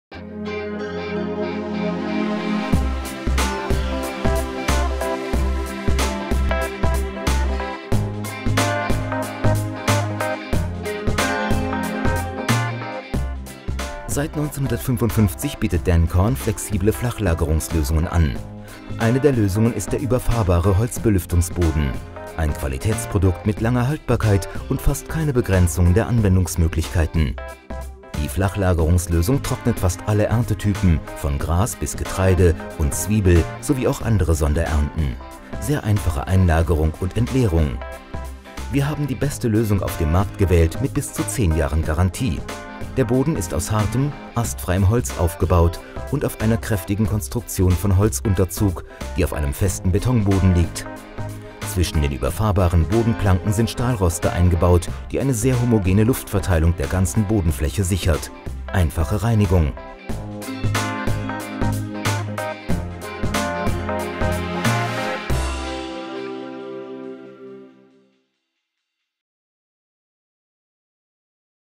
DANCORN Flachlagerung - Produktvorstellung - Deutsche Sprache
Seit 1955 bietet DANCORN flexible Flachlagerungslösungen an.
Eine der Lösungen ist der überfahrbare Holzbelüftungsboden. Ein Qualitätsprodukt mit langer Haltbarkeit und fast keine Begrenzungen der Anwendungsmöglichkeiten.
Die Flachlagerungslösungen  trocknen fast alle Erntetypen, von Gras bis Getreide und Zwiebel sowie auch andere Sonderernten. Sehr einfache Einlagerung und Entleerung.
Wir haben die beste Lösung auf dem Markt gewählt mit bis zu 10 Jahren Garantie.
Der Boden ist aus hartem astfreien Holz aufgebaut, und auf einer kräftigen Konstruktion von Holzunterzug, die auf einem festen Betonboden liegt. Zwischen den überfahrbaren Bodenplanken sind Stahlroste eingebaut, die eine sehr homogene Luftverteilung der ganzen Bodenfläche sichert. Einfache Reinigung.
Hier finden Sie einige gute Tipps über Getreidetrocknung, Turnkey-Lösungen, Projekte allgemein. Dan-Corn verfügt über 12.000m ² Lagerhallen in Hedensted.